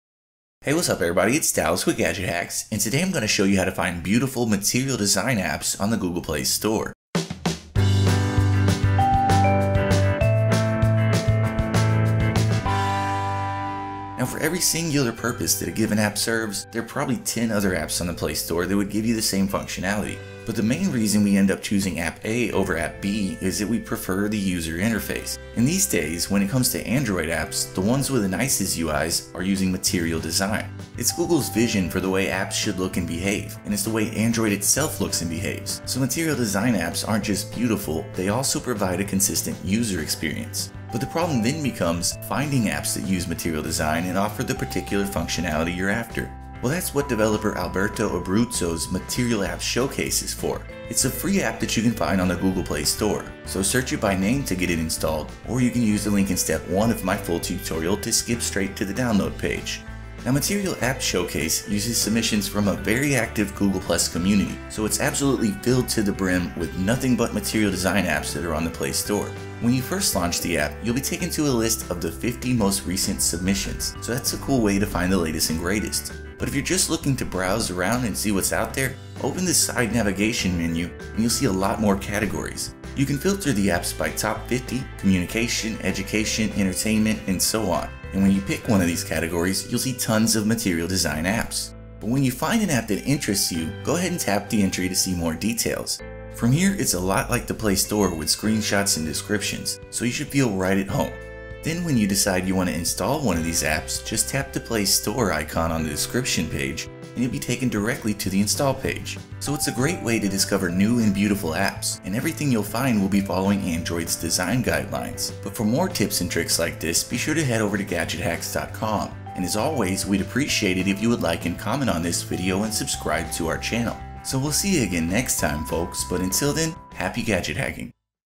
Find Beautiful Material Design Apps on the Google Play Store [How-To]
How to Get a Curated List of Only Material Design Apps [Android]

In this video, I'll be showing you a new Android app called "Material Apps Showcase" that uses submissions from a very active Google+ community to display all of the latest and greatest Material Design apps for Android.

With this one installed, you'll be able to find a Material substitution for pretty much any app on your phone, so you don't have to put up with ugly Gingerbread-era interfaces any longer.